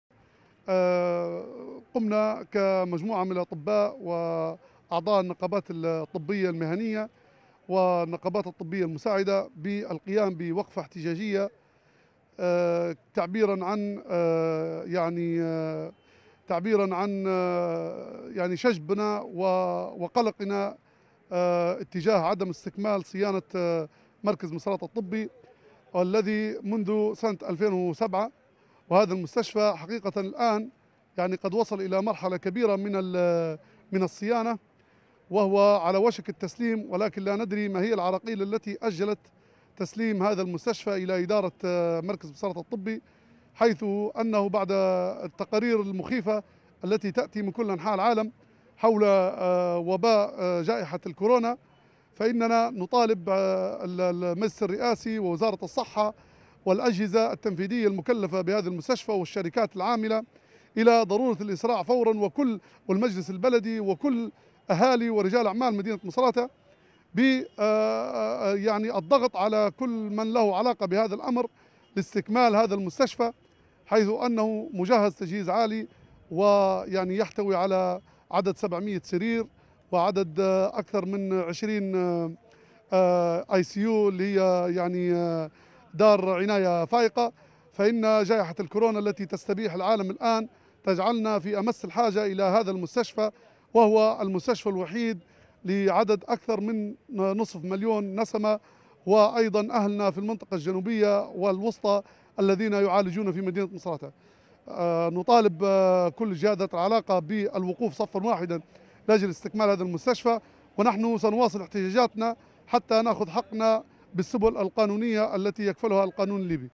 معن السويحلى : قمنا كمجموعة أطباء بالقيام بوقفة احتجاجية على عدم استكمال مركز مصراتة الطبي
معن السويحلى/ أحد المشاركين في حراك النقابات الطبية بمصراتة: قمنا كمجموعة أطباء بالقيام بوقفة احتجاجية على عدم استكمال مركز مصراتة الطبي الذي دخل الصيانة منذ 2007

ـــــــــــــــــــــــــــــــــــــــــــــــــــــــــــــــــــــــ

ــــــــــــــــــــــــــــــــــــــــــــــــــــــــــــــــــــــــــ
التناصح